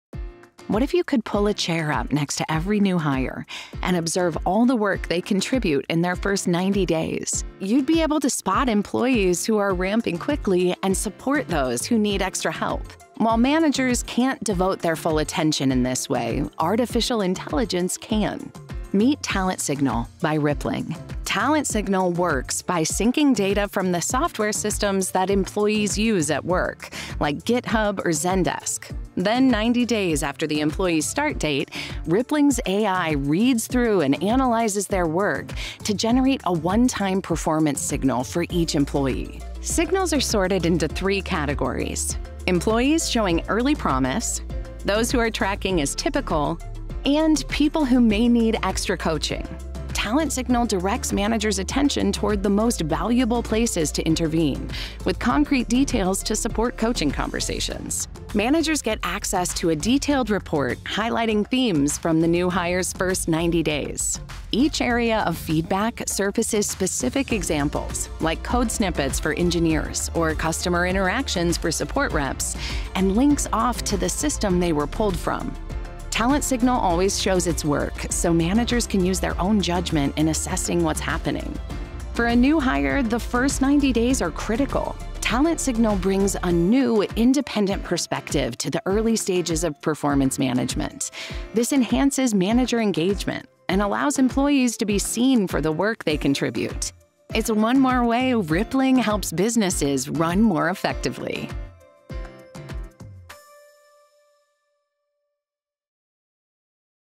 Meet Talent Signal: The AI helping new hires succeed | Rippling Product Launch
What if you could pull up a chair next to every new hire to review every line of code they write or each customer interaction they have, assess their first 90 days of work, and generate an independent report that allows you to provide them with helpful feedback?

Today, we’re launching Talent Signal, an AI-based talent analytics tool, to make this a reality.

It’s now available as an Early Access program for a group of existing Rippling customers.